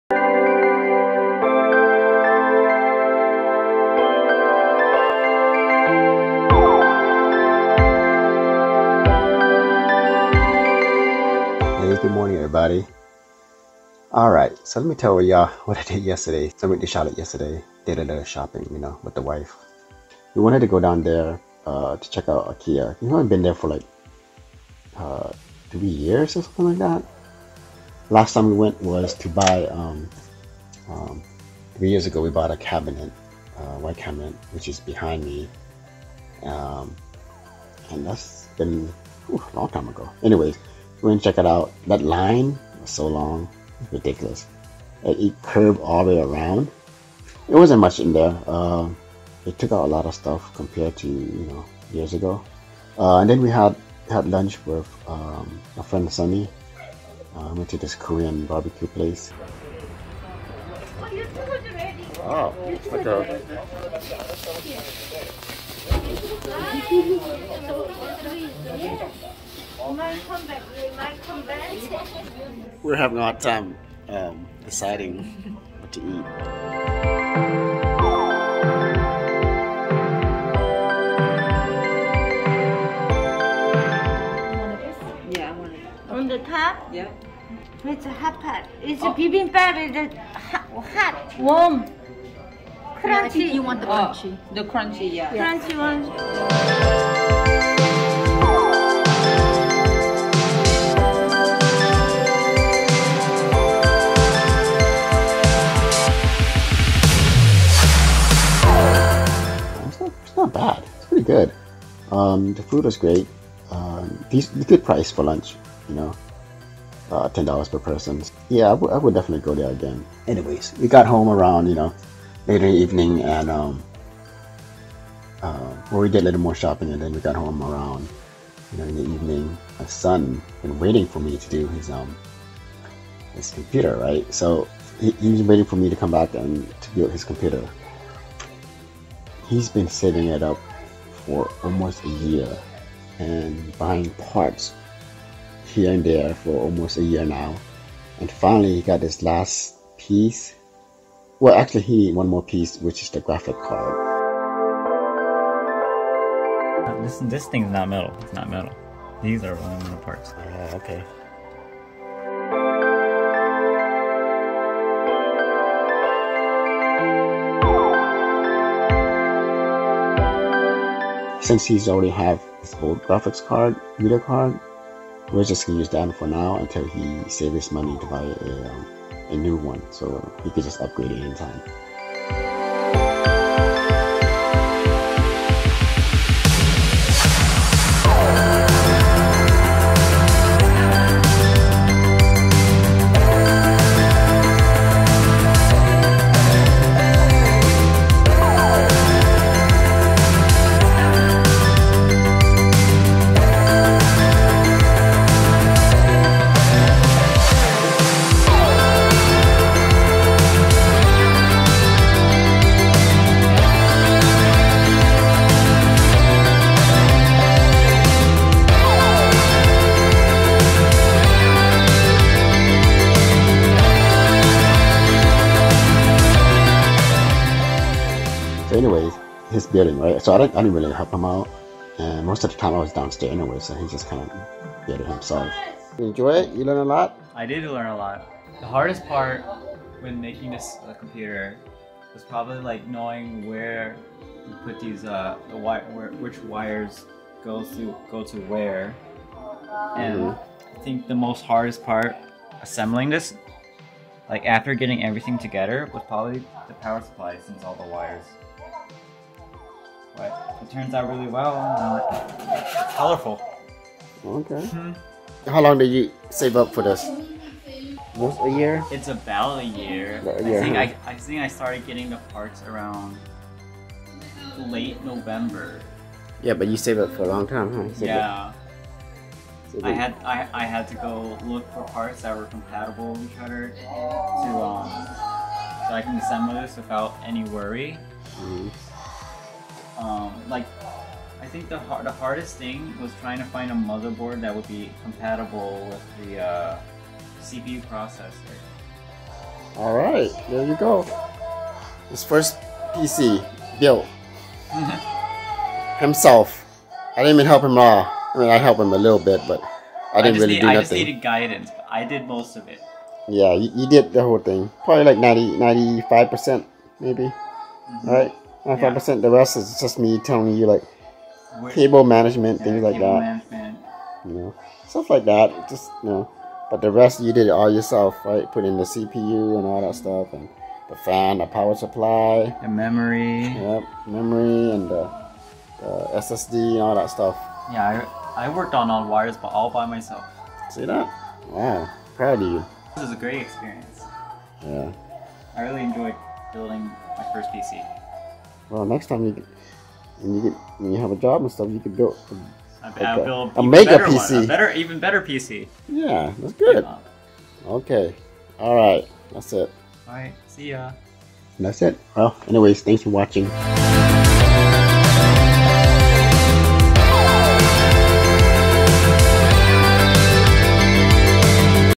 Weekend Vibes | Pepero Korean Restaurant | Ikea | Ryan first time building a PC | Charlotte NC
- Met up with our friend for lunch.
- Shop at Ikea for a bit
- Ryan first time building a PC